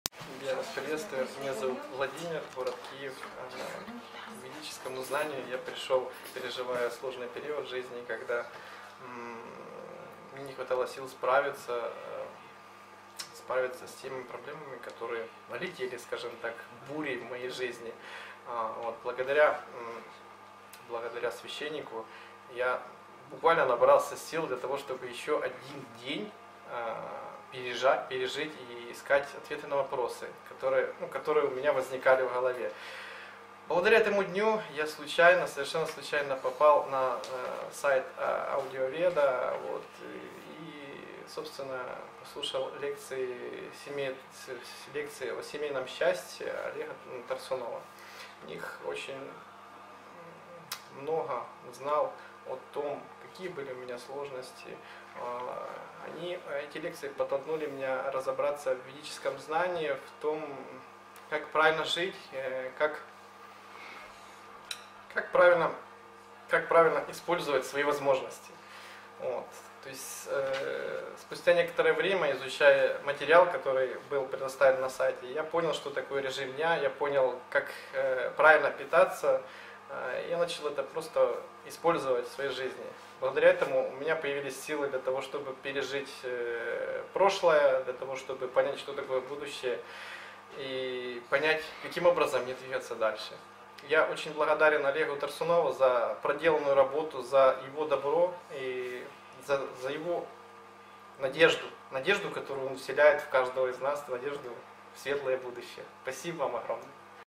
Отзыв Владимира о лекциях Олега Торсунова
В этом видео, вы можете посмотреть видео-отзыв Владимира из города Киева, о том как лекции Олега Торсунова повлияли на его жизнь.

Клуб "Благость" в г. Киев: Интервью с участником клуба Сергеем.